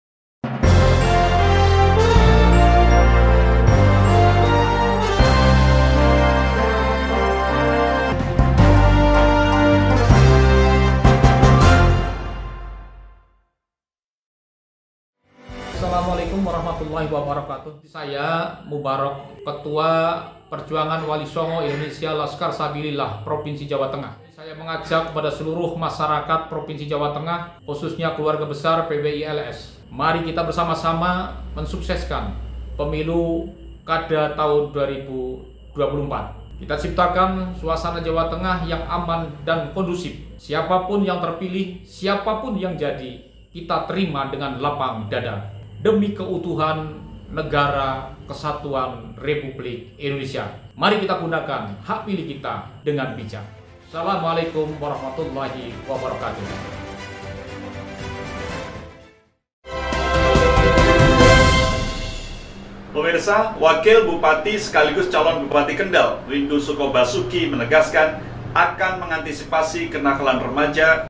KETUA PWI LASKAR SABILILLAH AJAK GUNAKAN HAK PILIH DENGAN BIJAK
KETUA PWI LASKAR SABILILLAH JAWA TENGAH, H. MUBAROK MENGAJAK MENCIPTAKAN SITUASI YANG KONDUSIF PADA PILKADA 2024. IA JUGA BERPESAN AGAR MASYARAKAT JAWA TENGAH MENGGUNAKAN HAK PILIH DENGAN BIJAK